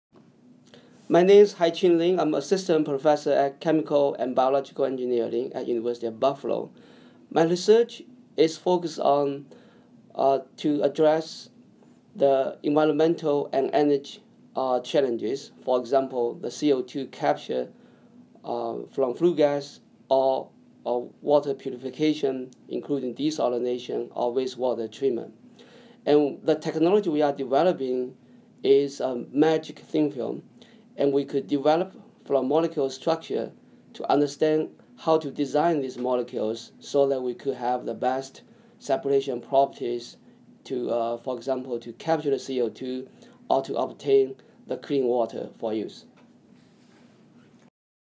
Haiqing Lin, PhD - What I Do
Assistant Professor of Chemical and Bio Engineering.  Research addresses environmental and energy challenges. Water Purification.